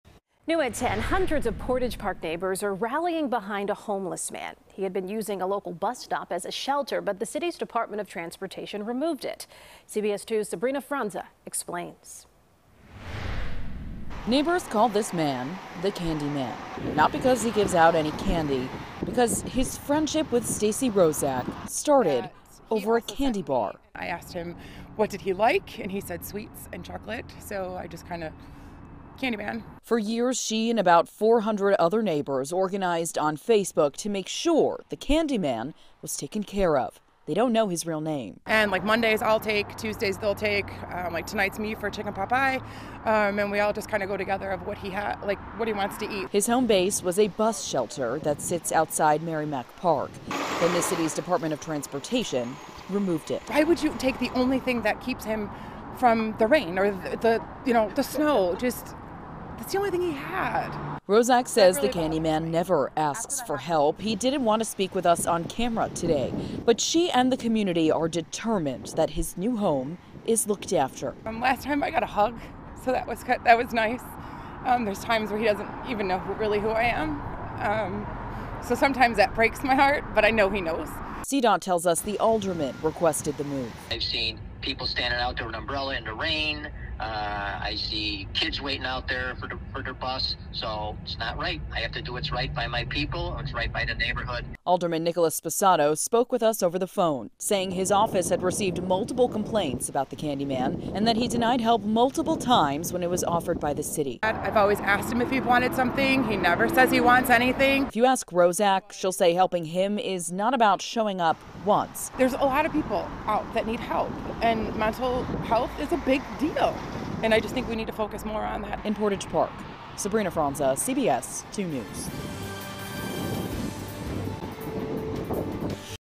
Portage Park neighbors rally around homeless man after CDOT removes bus shelter where he lived
The homeless man is known as the CandyMan not because he gives out any candy, but because his friendship with Stacy Roszak started over a candy bar. CBS 2's Sabrina Franza reports on the fight after his shelter was taken away.